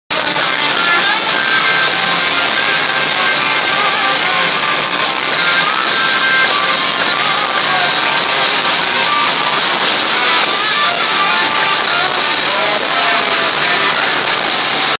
Billy Talent
Billy Talent in Moose Jaw sask canada on March 25 2013 @t the Mosaic Place. Sorry the muffled noise.